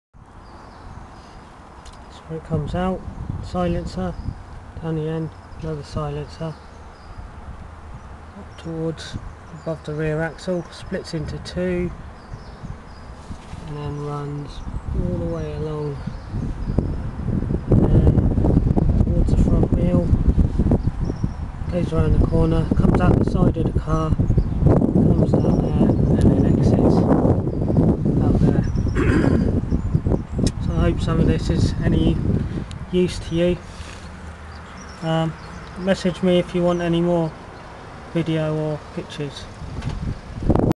Suzuki X-90 with AC Cobra Style Exhaust Discribed (5)
A pointing finger explanation of the system from the rear of the car